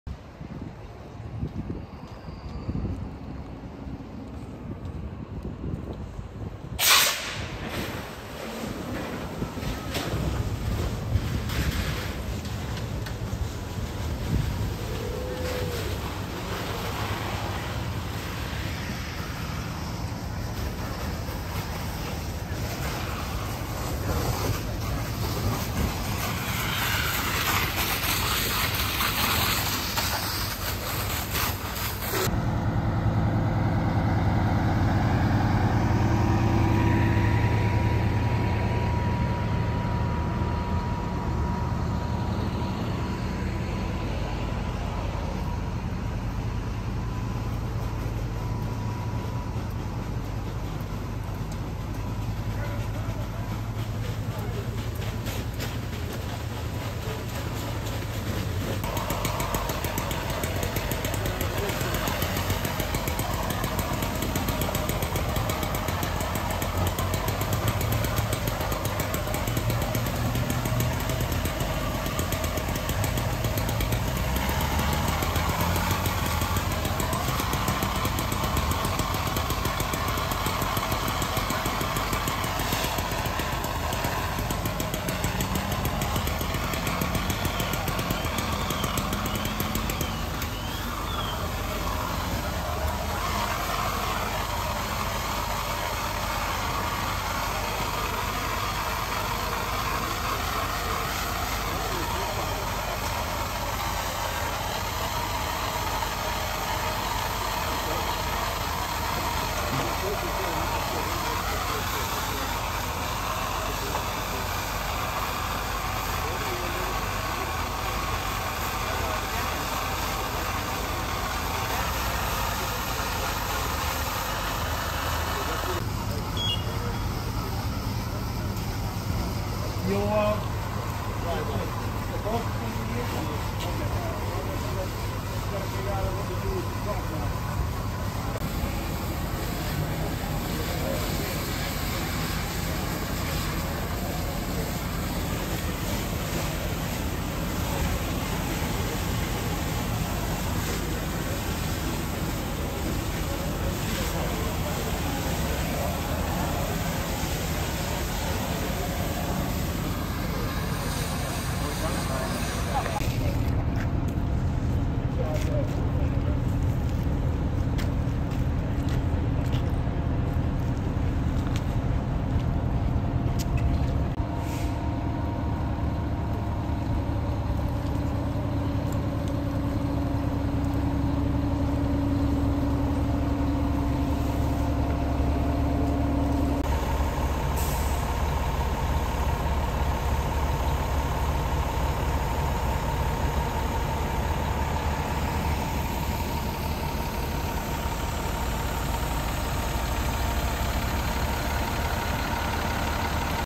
Mineola, New York Truck Fire
Mineola, New York
Truck fire at Station Rd and Third Ave. 3/2/23